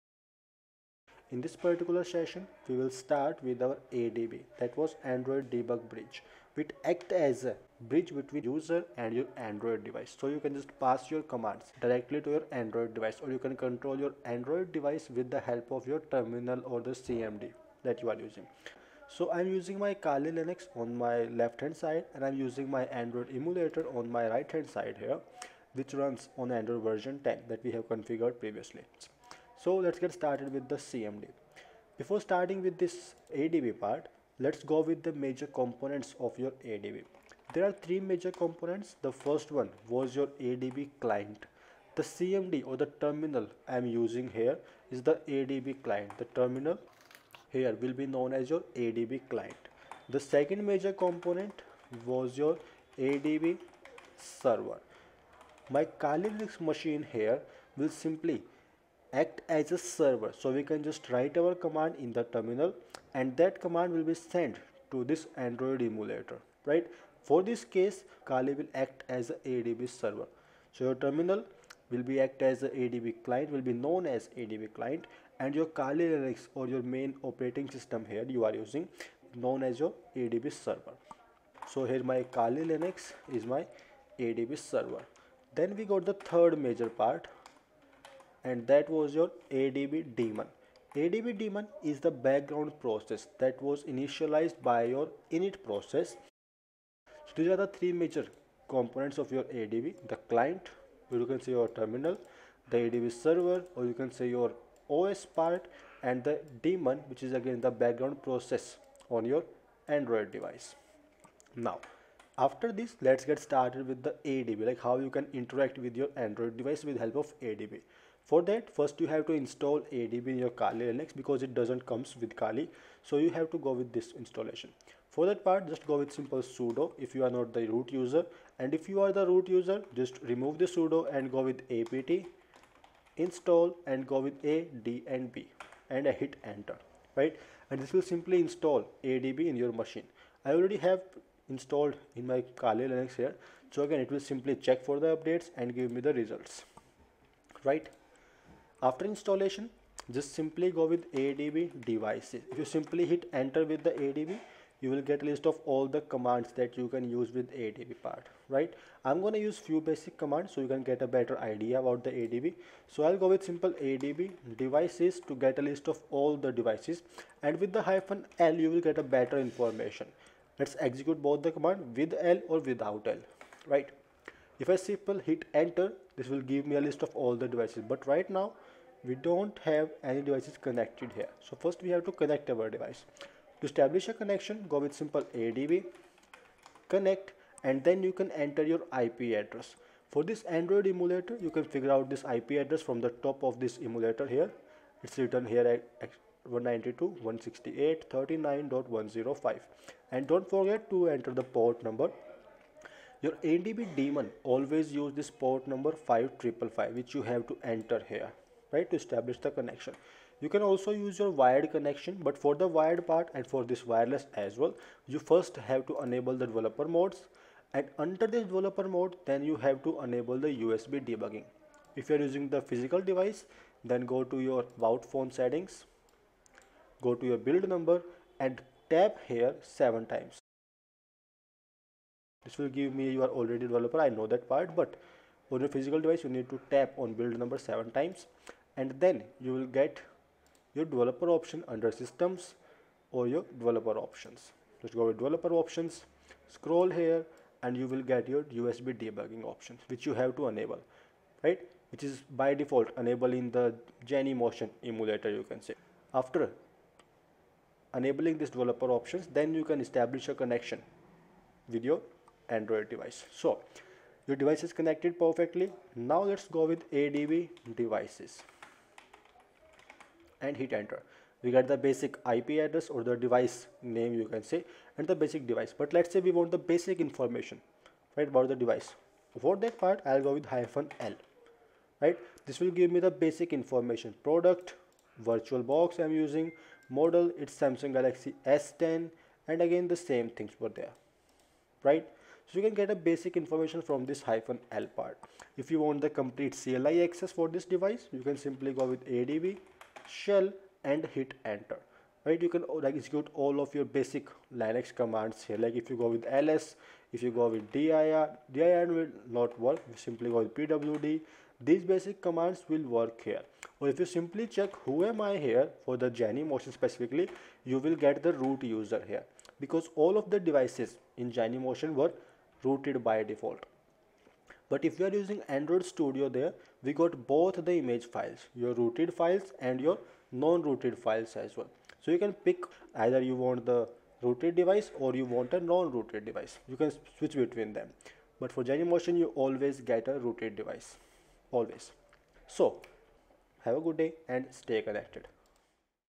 2.4 ADB (Android Debug Bridge)
Lets control our android device with ADBd (Android Debug Bridge daemon)